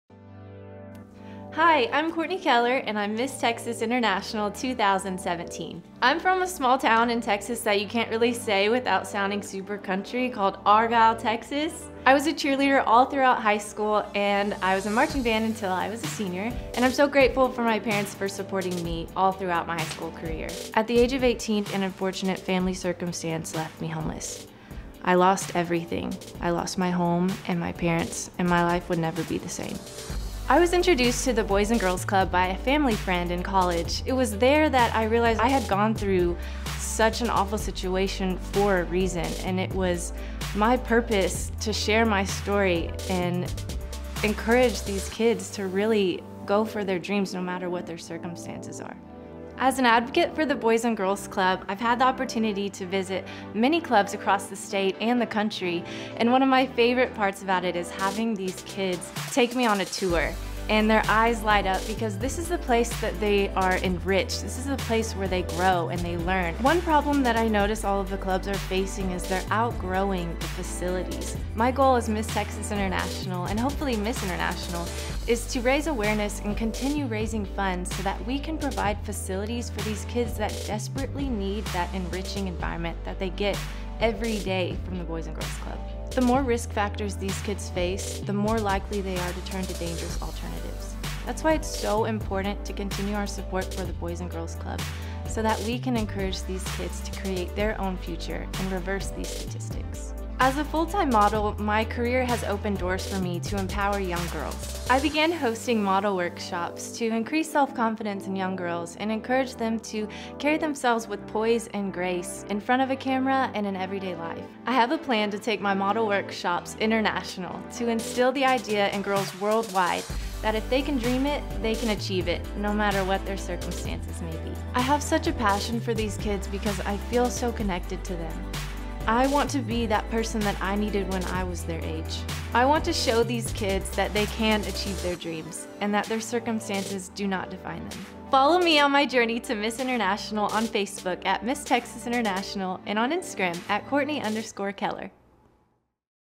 Journey To Miss International: Miss Texas International 2017
Meet Miss Texas International 2017 - Kourtney Kellar as she starts her journey to Miss International 2017!